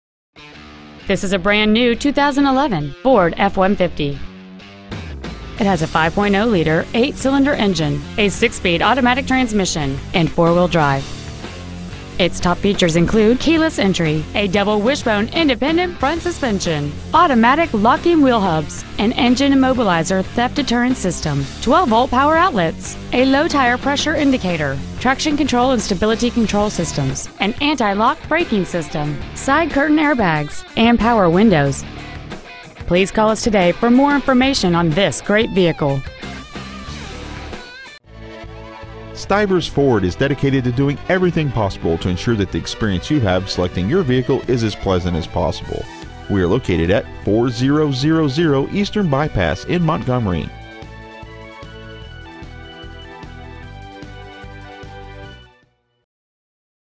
2011 FORD F-150 Montgomery AL
Year : 2011
 Make : FORD
 Model : F-150
 Engine : 5.0L V8
 Trans . : AUTO 6SPD
 Exterior : BLACK
 Miles : 22

 Stock : BF83794